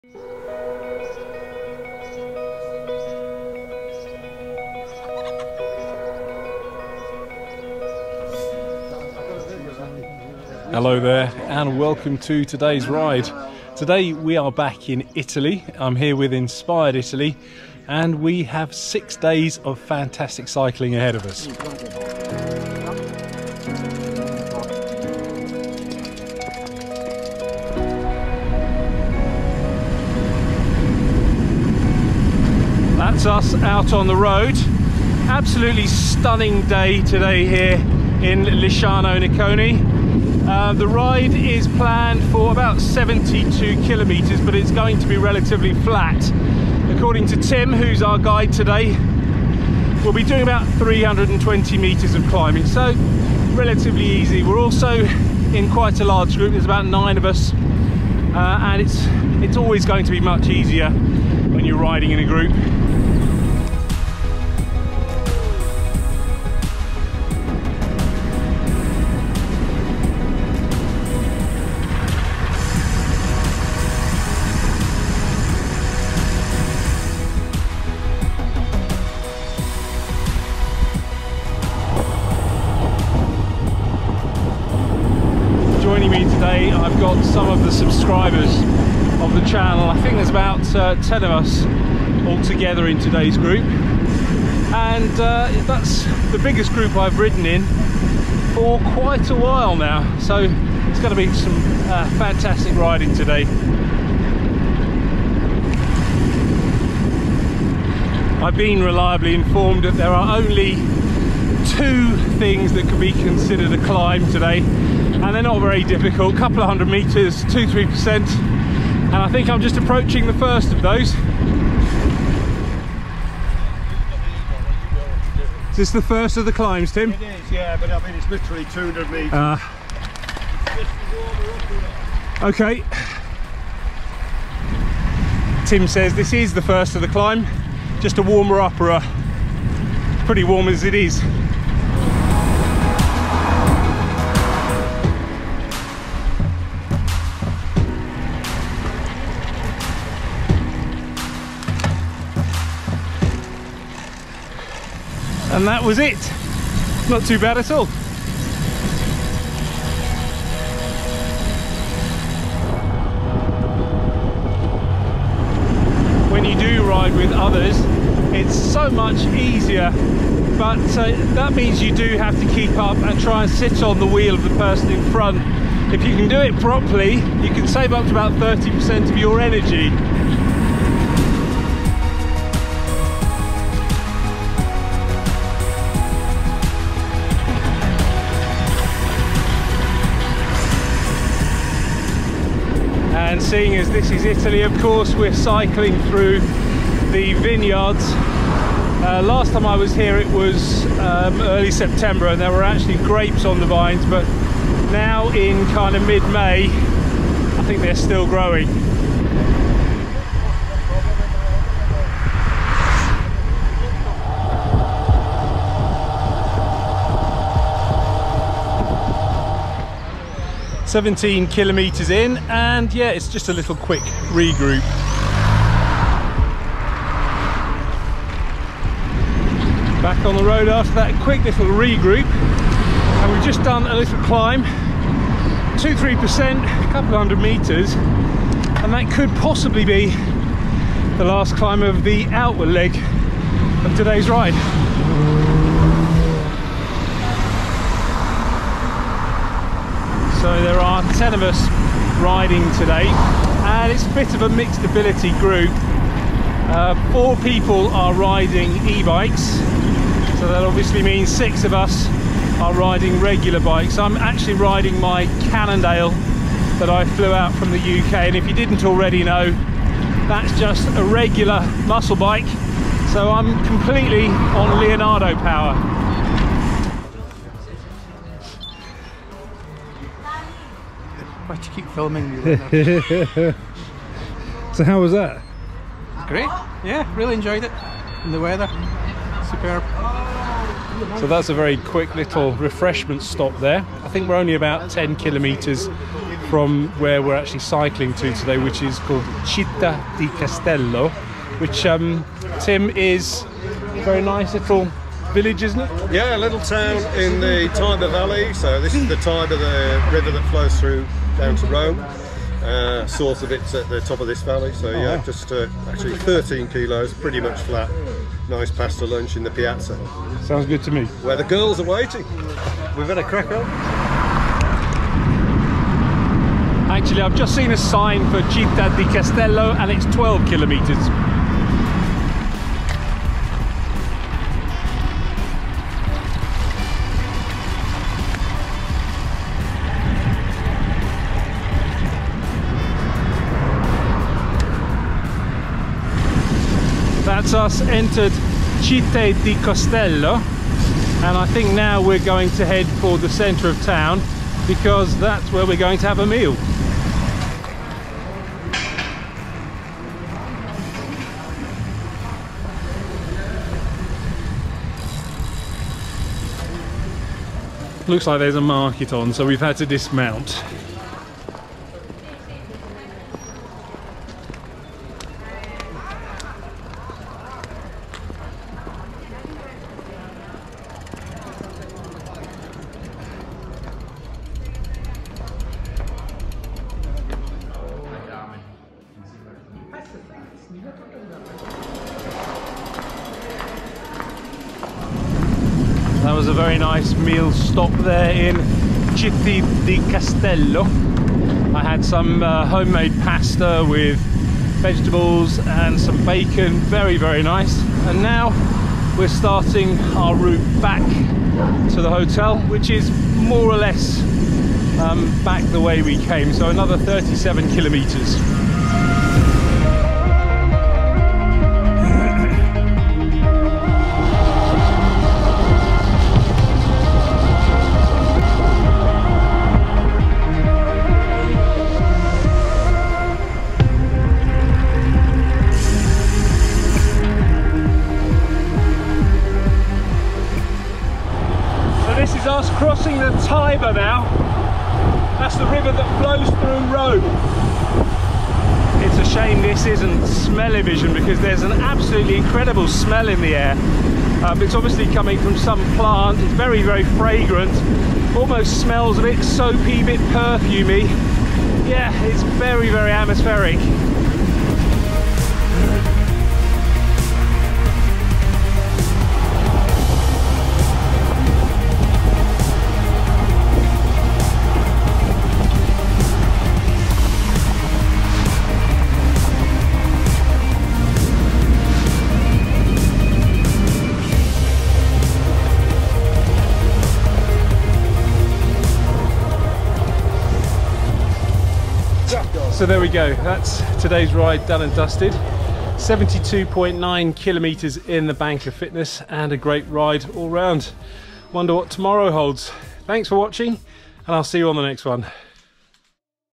A Glorious 75kms Ride On Italian Roads
A 75km ride on the roads of Umbria in Italy. It's also my first big group ride in quite a few years.